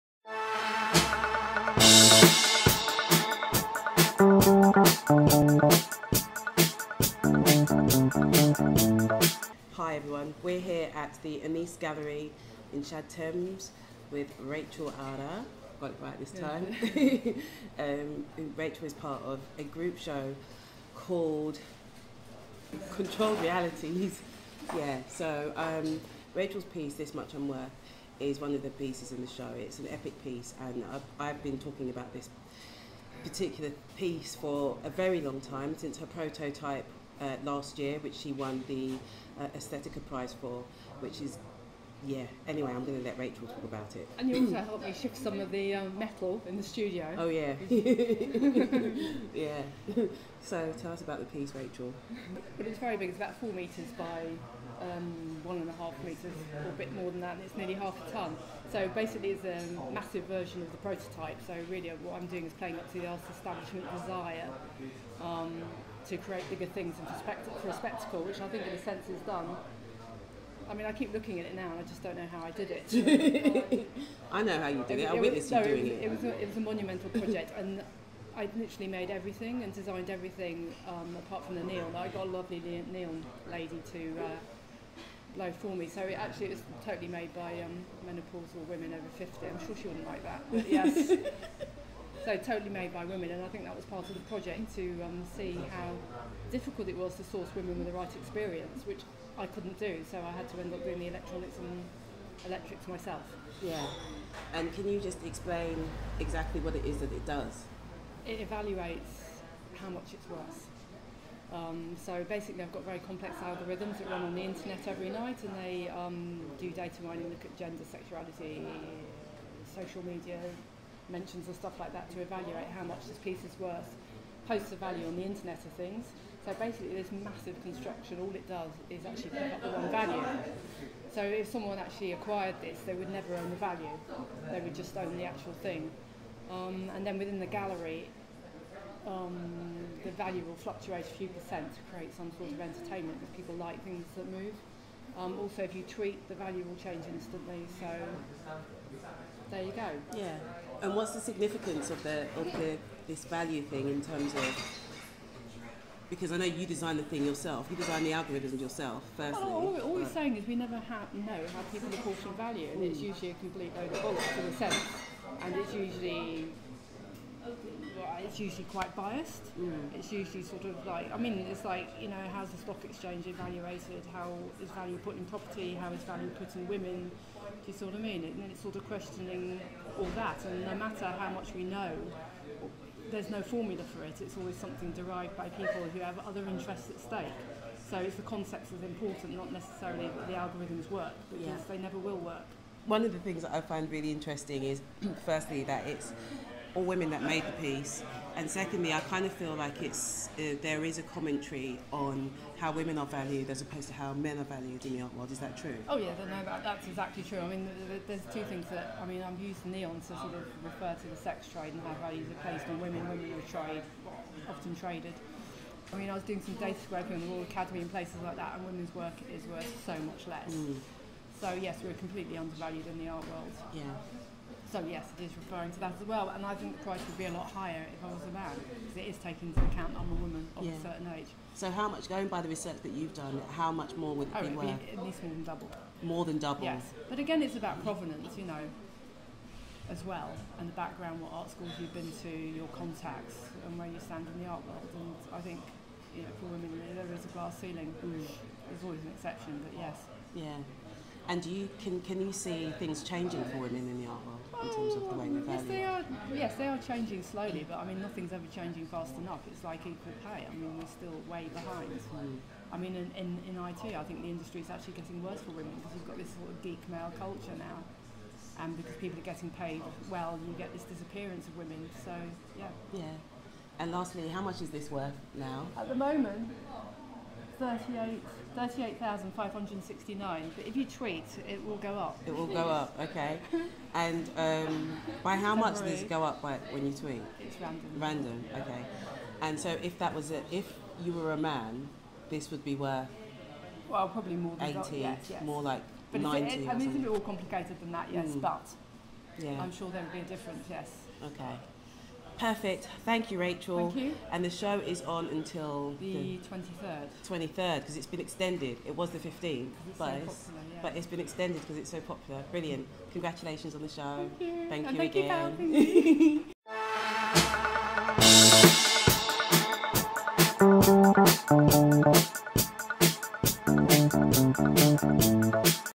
Rachel Ara Talks About "This Much I'm Worth": Art Discussion
Rachel Ara is a Conceptual and Data artist.  She gained her BA in Fine Art from Goldsmiths College, London where she won the prestigious Burston award for painting.   In 2016 she won the International Aesthetica art prize for her digital sculpture "This Much I'm Worth" .  She currently lives and works in London.

She has had a successful 25 year career as computer systems designer and is also a trained cabinetmaker.  In 2013 she started her professional art practice.

Rachel combines her manual skills with her programming and analytical ones  to create nonconformist artworks with a socio-political edge that often incorporates humor and irony.

Her most recent work is a large scale version of the award winning work, the self-evaluating artwork, that uses complex algorithms to calculate its own value. A spectacular four dimensional piece 4m x 2m, constructed from 72 pieces of neon, electronics, IP cameras, recycled server room equipment and circuit boards that communicate information back and forth to the Internet.  Other recent works include CAD drawings inspired by scenes of gender based violence, D.O.A.M III, that  was a finalist for the 2016 Lumen Prize and International tour.

She is currently working towards a 3 person show, “Controlled Realities” at the Anise Gallery London., amongst other projects.

About "This Much I'm Worth":
The artwork is connected to the internet and so becomes part of the world wide web.  Feeding galley and location data via its sensors to the Internet (Internet of Things) and extracting and displaying its calculated value.

Complex algorithms called "endorsers" run nightly on external servers using the data gathered together with extensive data mining from the media and  internet to reevaluate the work.  The endorsers then post the new value back to the Internet of Things overnight where it is then retrieved and displayed by the artwork.

This piece will openly show its value, but the algorithm that the “endorsers” use is not public knowledge.

I like to see this work as ongoing research,  as time changes, data gathers, more analysis is done and questions change.

Nothing is fixed.

In terms of aesthetics, the neon design for the piece was reproduction of a  sex shop sign in Soho, London .. which also draws on my concerns about the value placed on women in our society.

Nixie lights were used for the digits on the prototype.  They are very visually engaging lights that are constructed from cold cathode tubes with stacked digits. The main concept behind the use of Nixie lights came from their use in old calculation devices.  Nixie lights were probably last broadcast in the countdown sequence of the famous 70s TV series “The six million dollar man”.

In terms of construction – I tend to construct my own work instead of outsourcing ie.  I soldered all the circuit boards, programmed the endorsers and microprocessors etc.…  It took about 4 months to construct with lots of tests and prototyping.  It’s a very international hybrid construction with components  sourced from parts all over the world including  USA, Ukraine, France, UK and China.